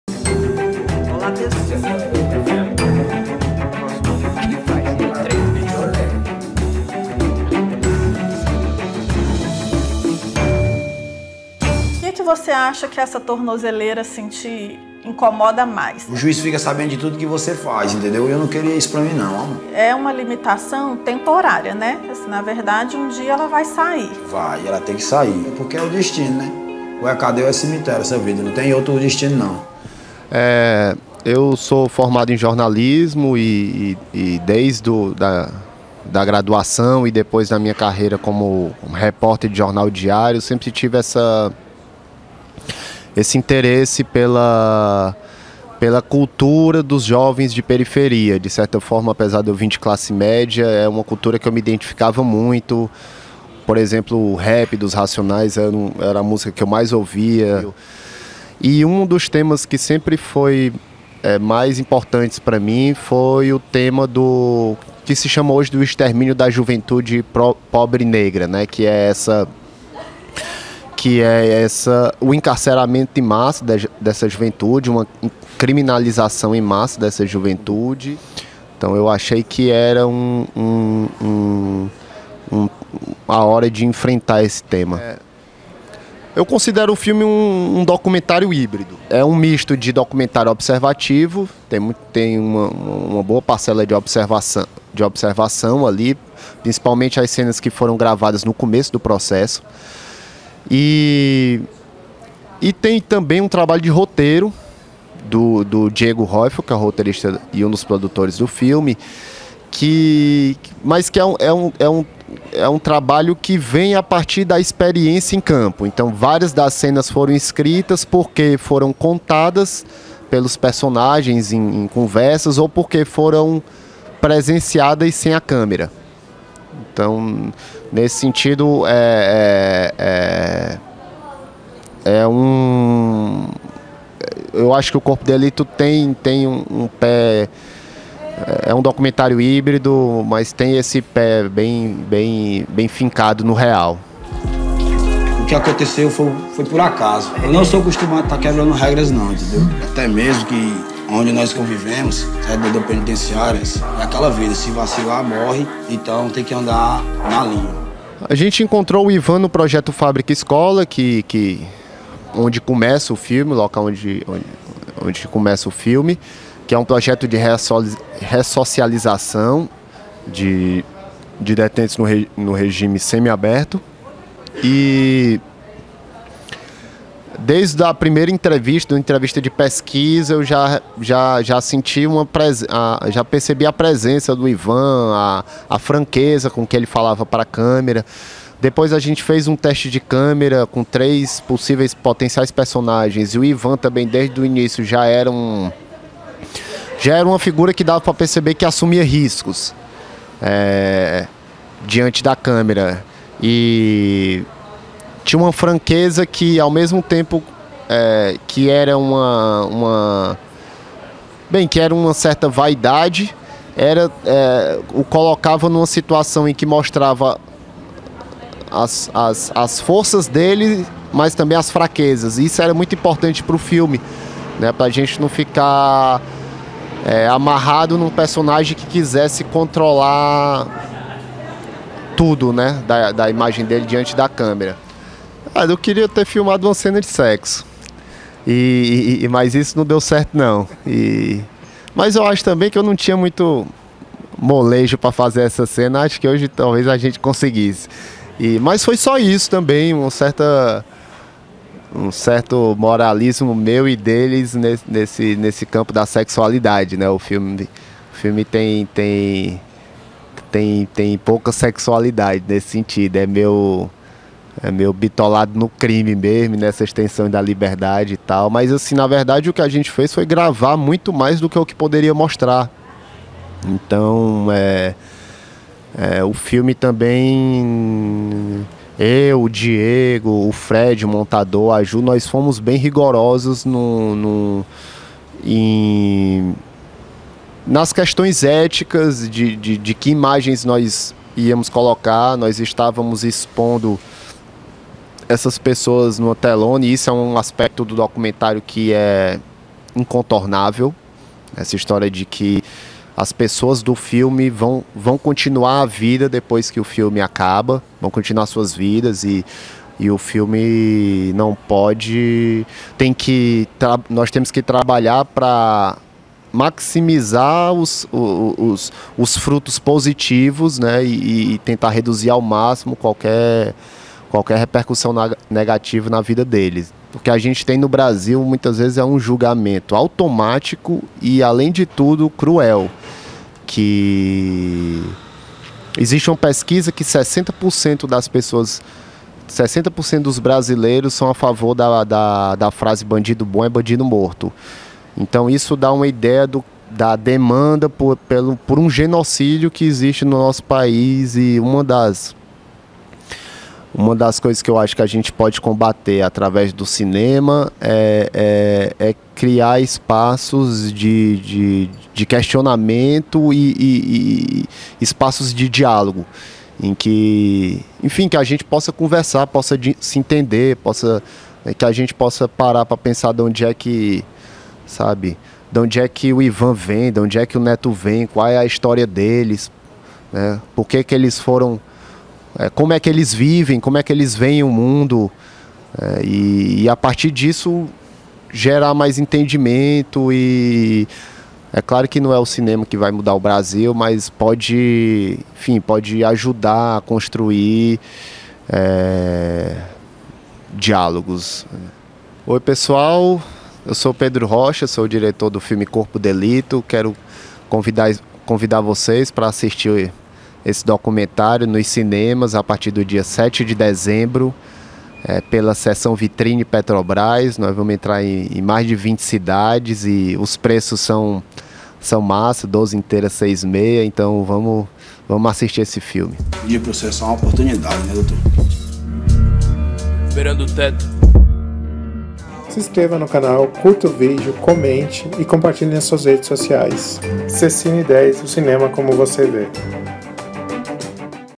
Pedro Rocha fala sobre o filme Corpo Delito
O diretor Pedro Rocha fala sobre o filme Corpo Delito